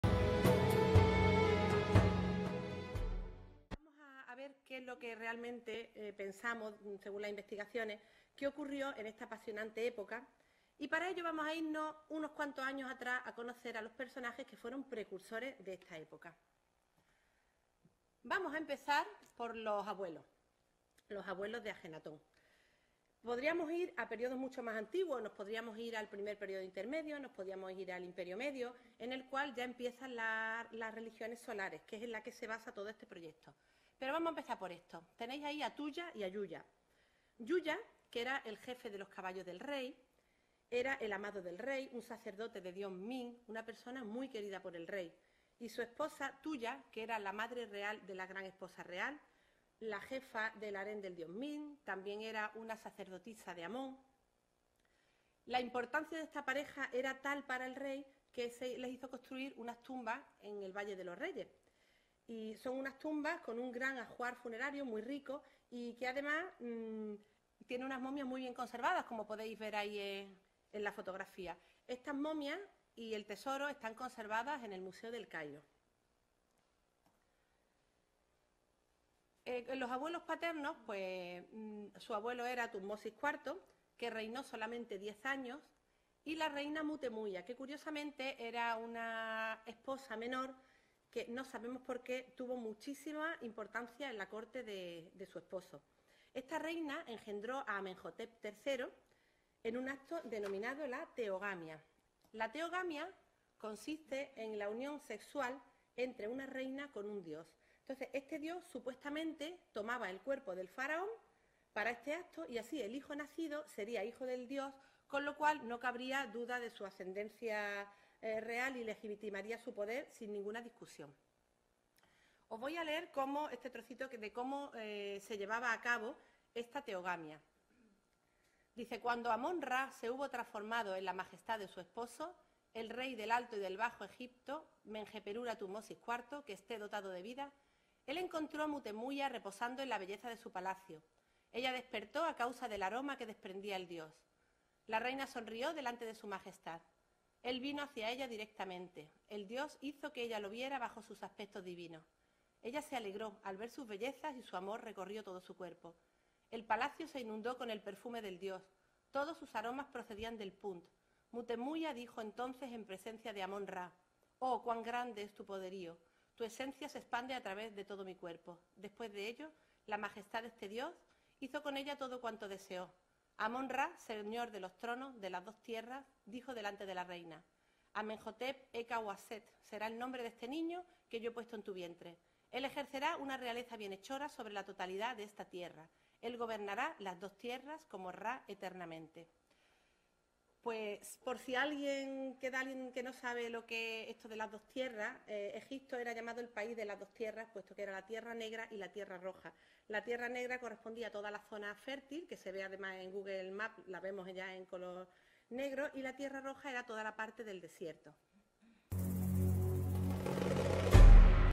Los Abuelos de Akhenaton (Conferencia sobre Amarna I parte)
Como ya os dije, que desglosaría la conferencia por si alguien prefería verla a trozos y que no resultase pesada, aquí tenéis la primera parte. Un breve vídeo sobre los abuelos de Akhenaton que nos pone en camino para todo lo  que viene después.